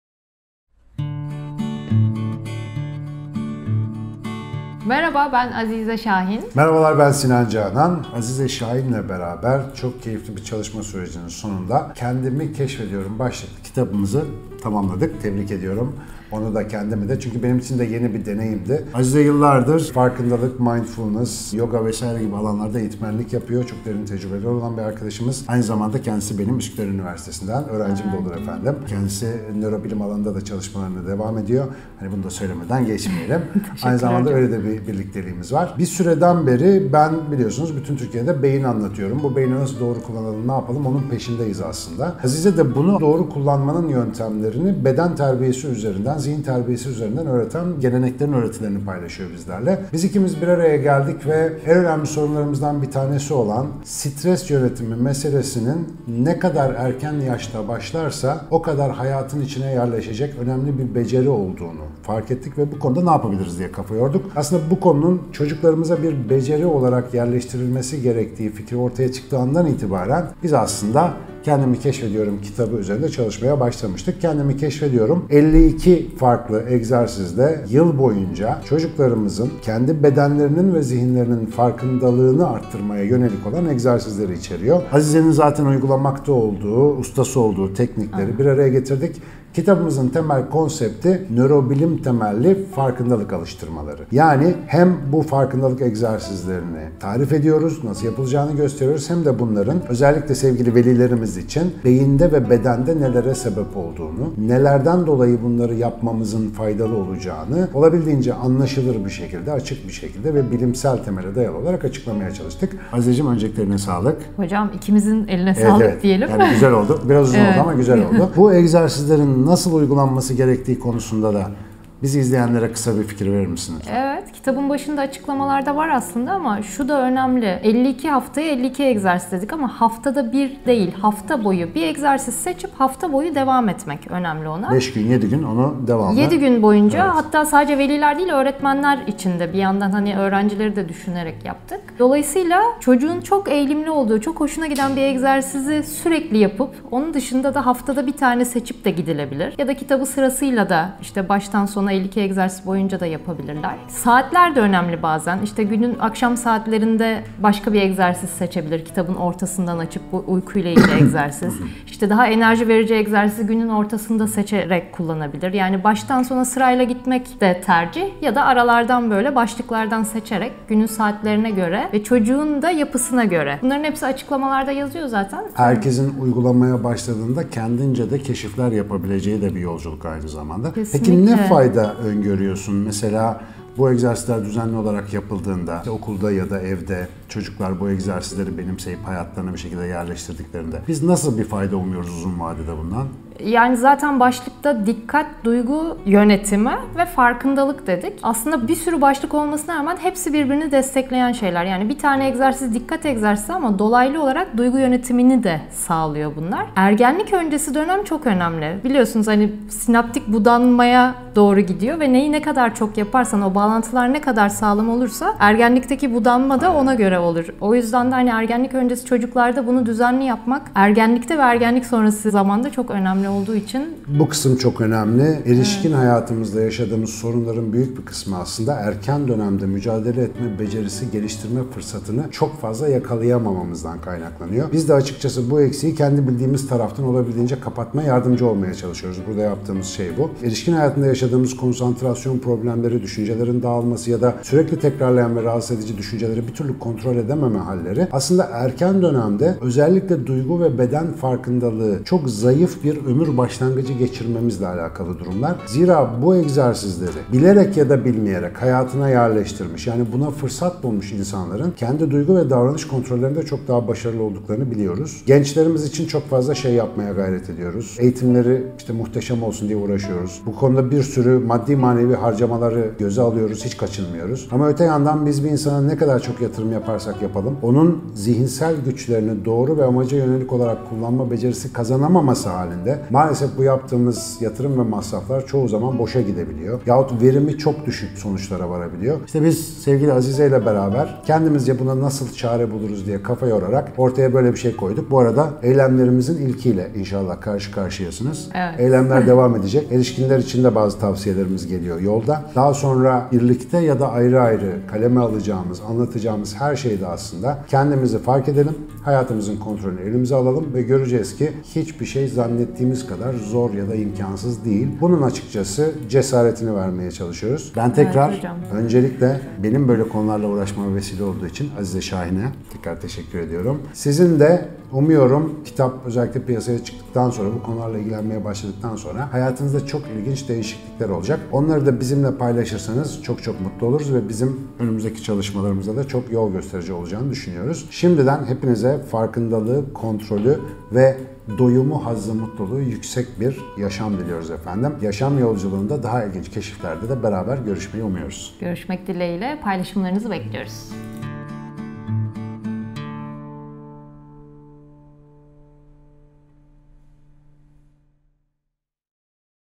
KENDİMİ KEŞFEDİYORUM - Kitap Tanıtımı / Sinan Canan & Azize Şahin
Prof. Dr. Sinan Canan ile Azize Sahin, "Kendimi Keşfediyorum"u sizler için anlattı. Özellikle çocuklara ve gençlere yönelik duygu, dikkat ve farkındalık egzersizleriyle beyinleri açan bu yeni kitabımızı tüm kitapçılarda ve aşağıdaki linkte bulabilirsiniz!